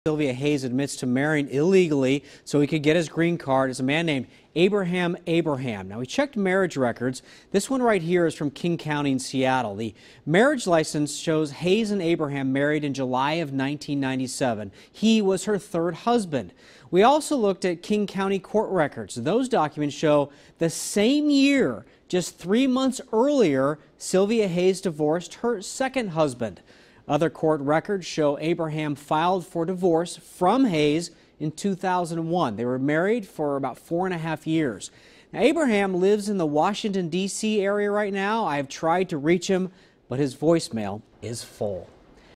The man Cylvia Hayes married in 1997
Court records show Cylvia Hayes married a man named Abraham Abraham in 1997 in order to help him obtain US citizenship